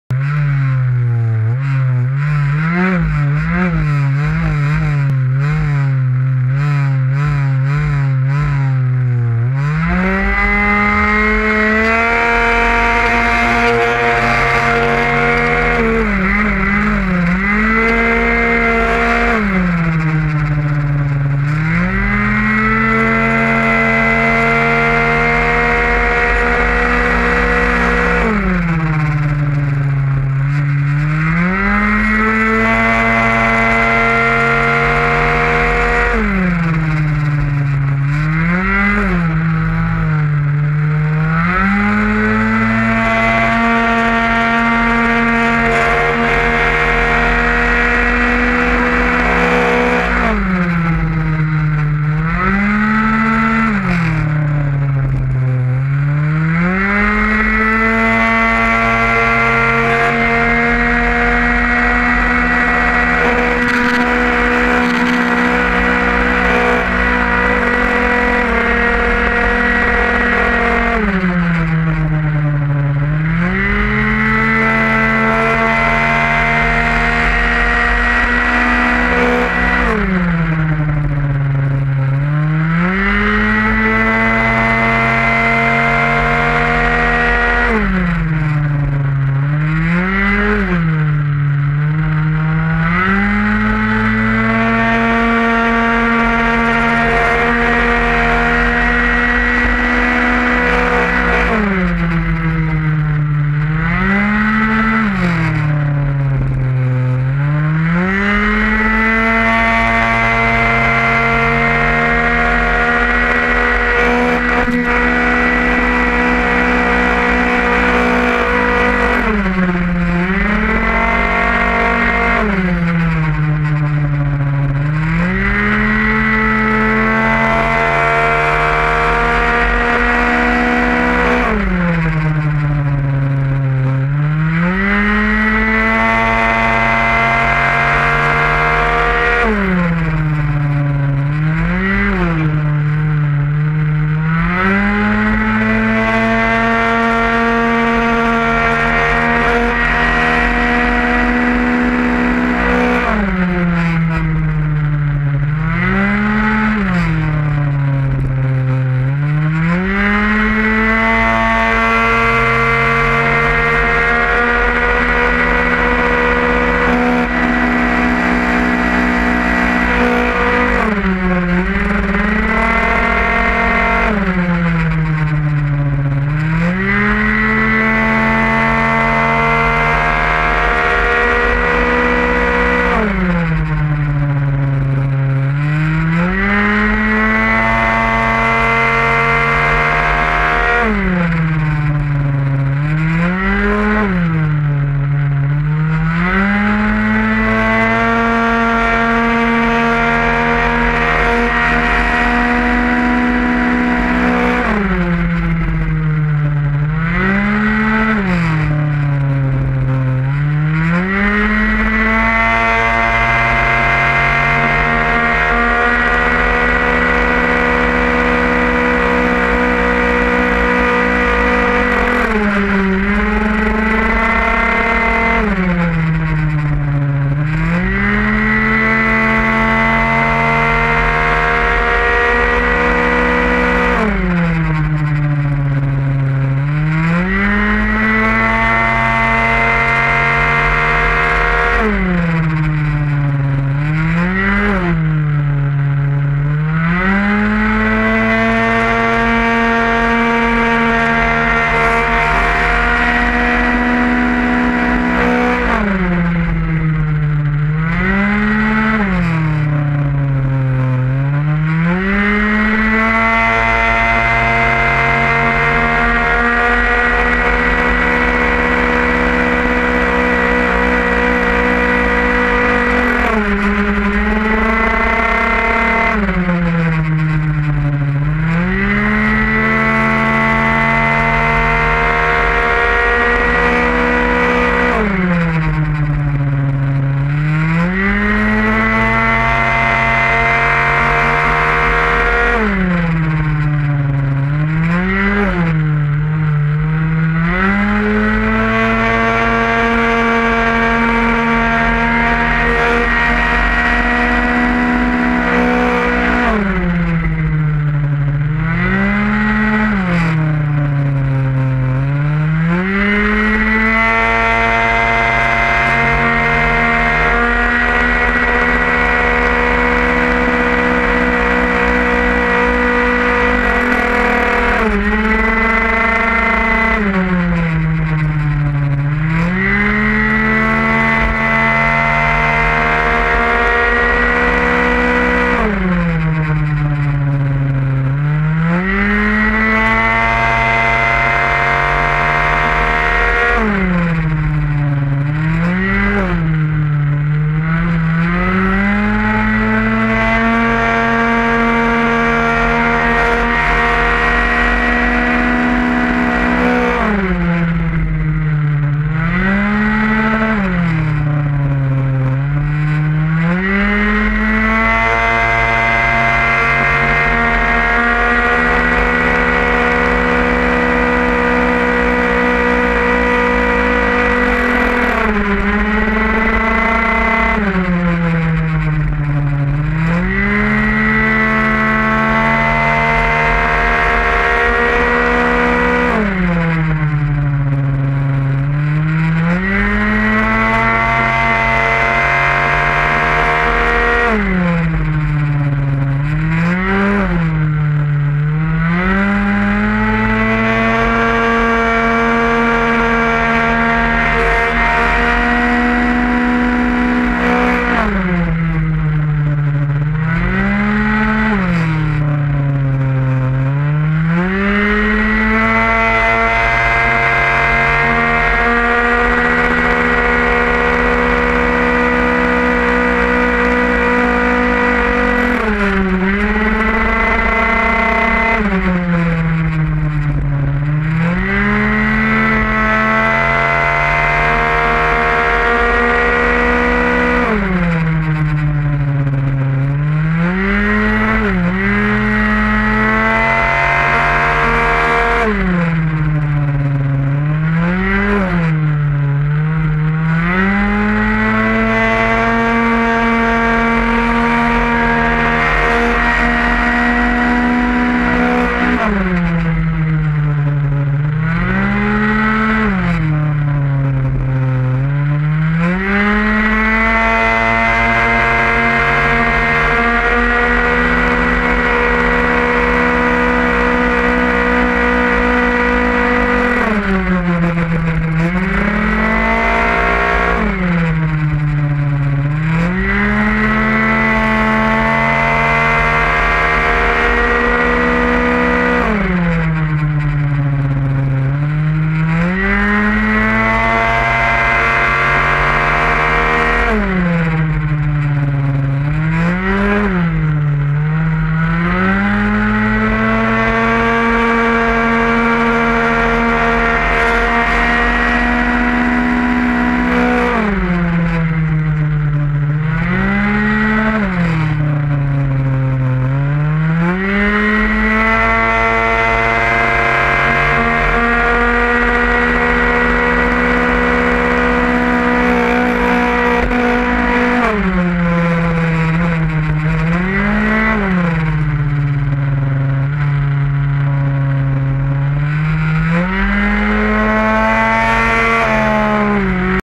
Manche 1 TGO GoPro Arrière Promo Scoot 50c#72 Circuit de Vendeuvre Dimanche 24 Juillet 2016
Pilote Julien Mach G #72 Promo Scoot 50cc Metrakit